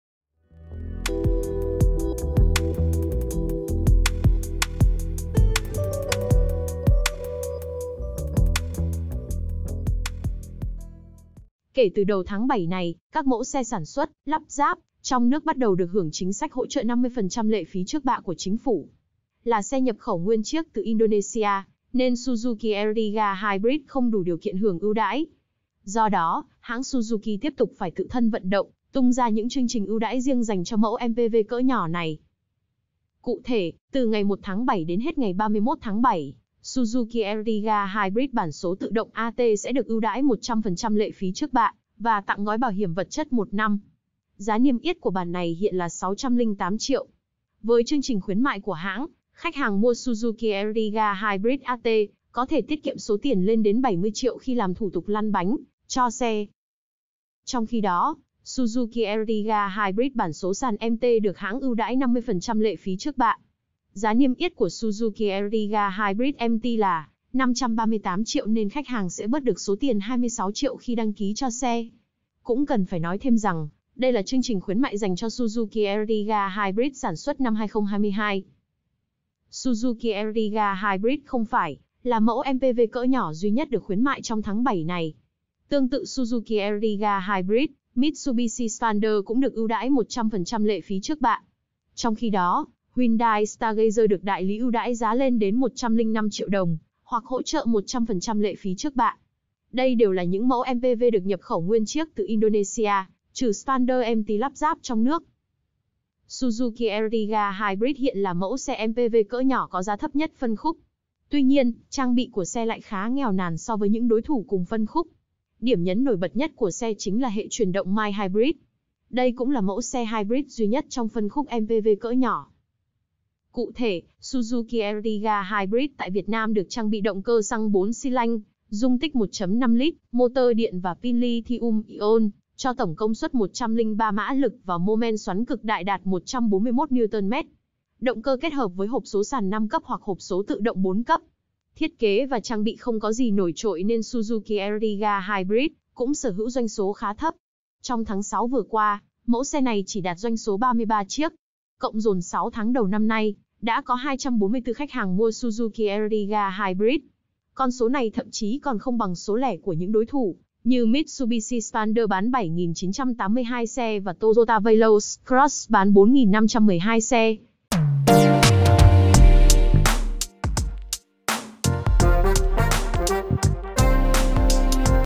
Thêm MPV 7 chổ có giá 500 triệu rẻ nhất Việt Nam sau Hyundai Stargazer khi được đại lý giảm 70 triệu
Suzuki Ertiga Hybrid bản số sàn MT được hãng ưu đãi 50% lệ phí trước bạ. Giá niêm yết của Suzuki Ertiga Hybrid MT là 538 triệu đồng nên khách hàng sẽ bớt được số tiền 26 triệu đồng khi đăng ký cho xe. Cũng cần phải nói thêm rằng, đây là chương trình khuyến mại dành cho Suzuki Ertiga Hybrid sản xuất năm 2022.